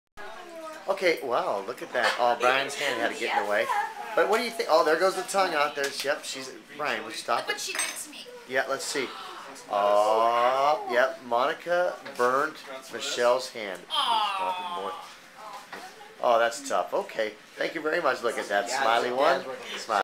Monica & Michelle
Michelle accusing Monica of burning her hand with a curling iron...